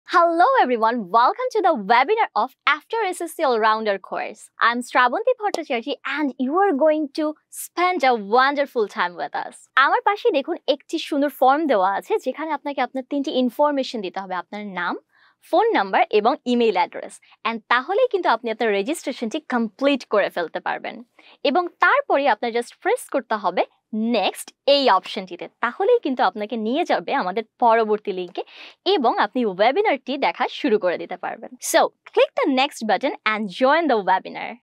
After SSC All-Rounder Course | Webinar | TESOL KIDS CENTER | TESOL BANGLADESH
Description:
Are you a student who has recently completed your SSC exams and looking to make the most of your time? TESOL Bangladesh presents an exciting webinar on the "After SSC All-Rounder Course." This webinar will provide you with valuable insights and guidance on how to excel in various aspects of personal development and academic excellence.